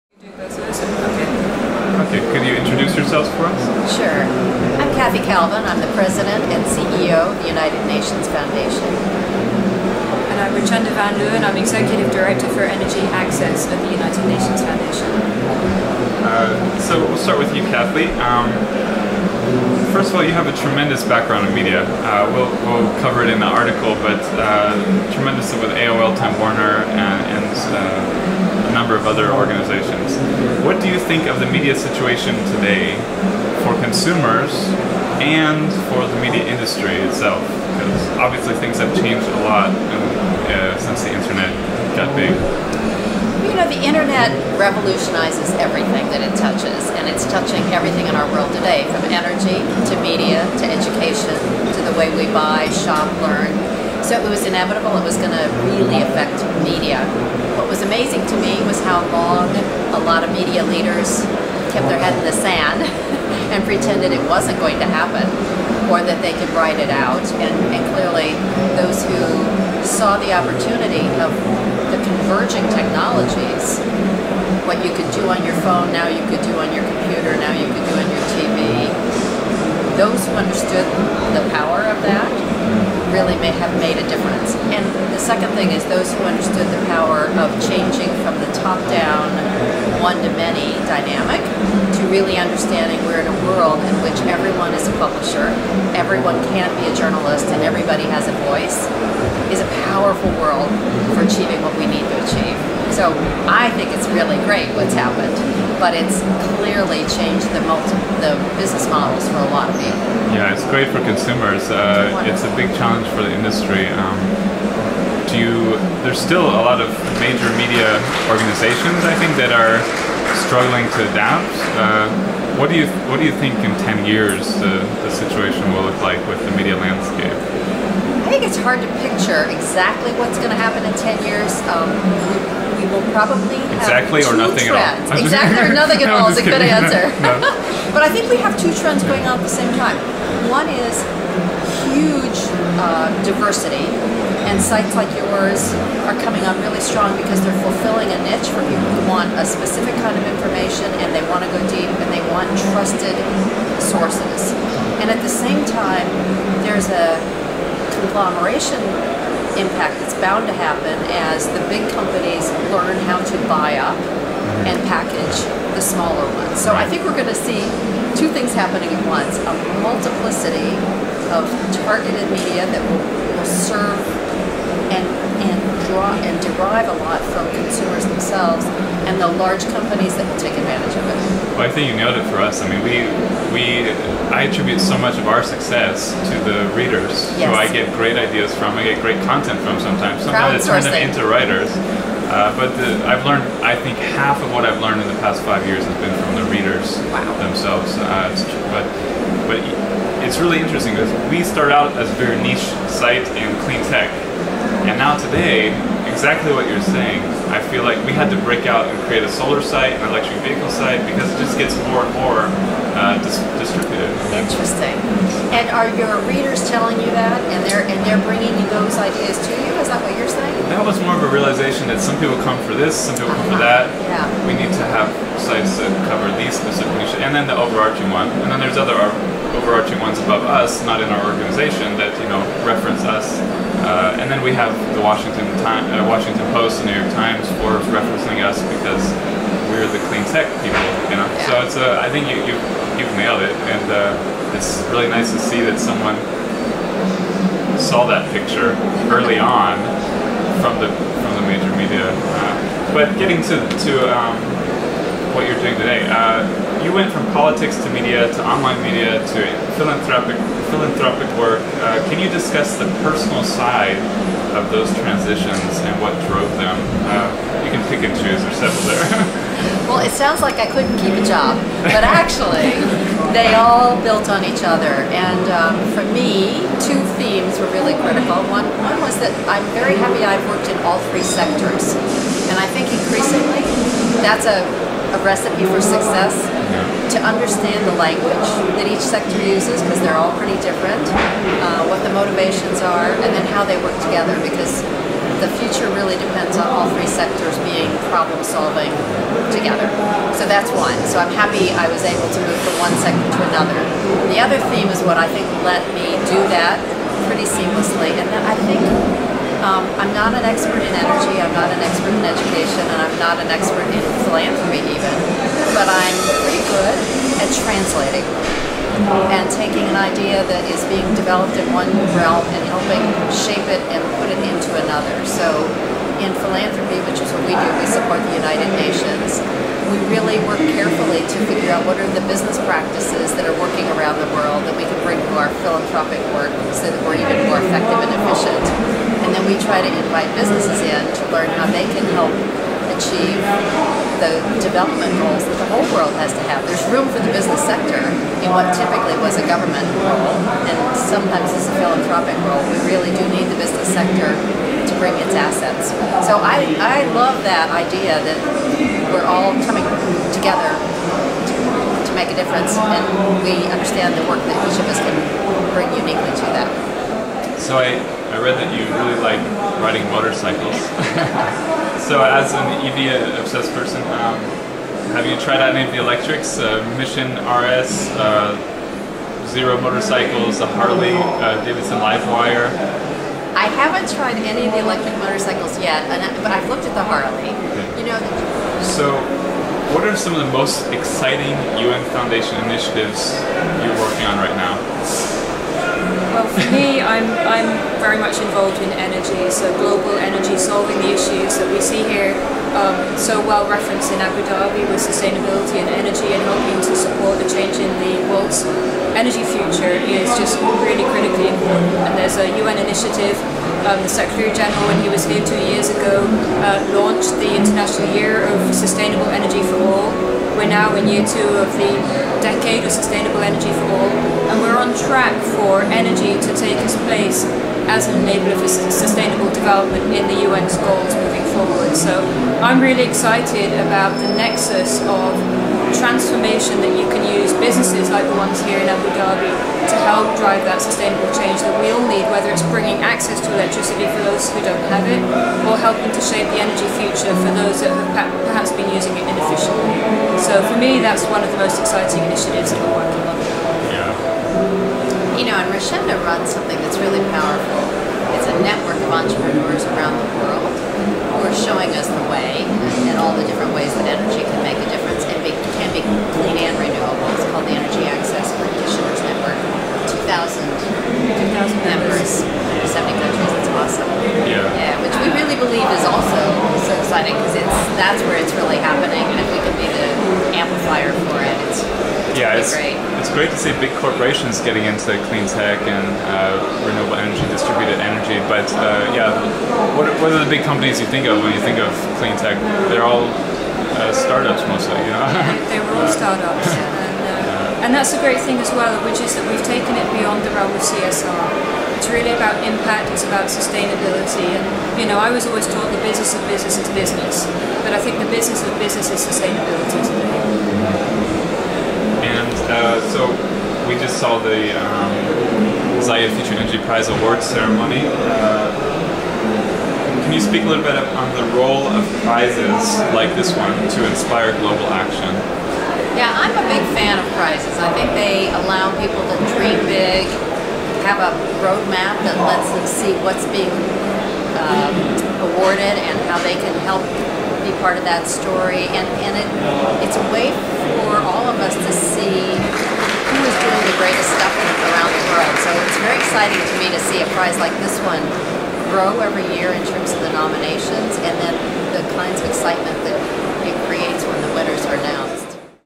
UN Foundation's Kathy Calvin and Richenda van Leeuwen - Interview
Interview with UN Foundation CEO and President Kathy Calvin, and Richenda van Leeuwen, UN Foundation Executive Director for Energy and Climate as well as Energy Access